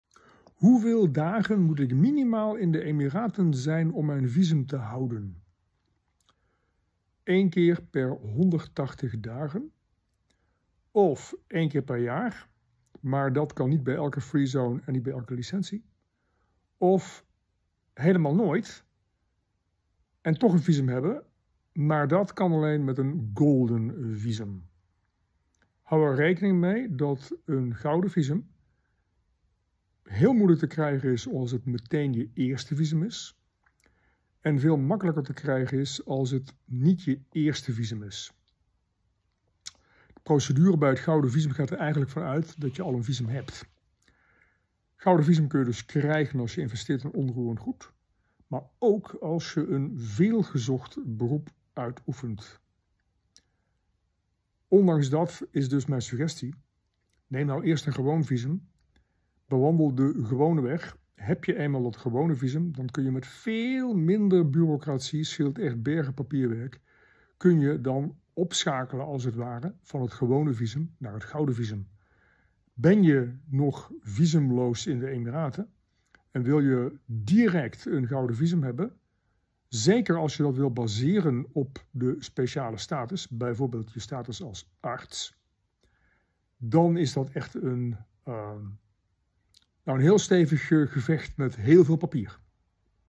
Een bedrijf starten in Dubai: Hoeveel dagen per jaar moet je minimaal in Dubai zijn?
Hoeveel dagen moet ik minimaal in de Emiraten zijn om mijn visum te houden?
Één keer per 180 dagen of één keer per jaar, maar dat kan niet bij elke Freezone en niet bij elke licentie, of helemaal nooit en toch een visum hebben, maar dat kan alleen met een GoldenVisum. Houd er rekening mee dat een Gouden Visum heel moeilijk te krijgen is als het meteen je eerste visum is, en veel makkelijker te krijgen is als het niet je eerste visum is.
De procedure bij het Gouden Visum gaat er eigenlijk van uit dat je al een visum hebt. Een Gouden Visum kun je dus krijgen als je investeert in onroerend goed, maar ook als je een veelgezocht beroep uitoefent.
Ondanks dat, is dus mijn suggestie neem nou eerst een gewoon visum, bewandel de gewone weg. Heb je eenmaal het gewone visum dan kun je met veel minder burocratie, scheelt echt bergen papierwerk, kun je dan opschakelen als het ware van het gewone visum naar het Gouden Visum.

Ben je nog visumloos in de Emiraten en wil je direct een Gouden Visum hebben, zeker als je dat wil baseren op de speciale status, bijvoorbeeld je status als arts, dan is dat echt een, nou een heel stevig gevecht met heel veel papier.

Wil je meer weten over ondernemen in de UAE?  Neem dan contact op met Paul via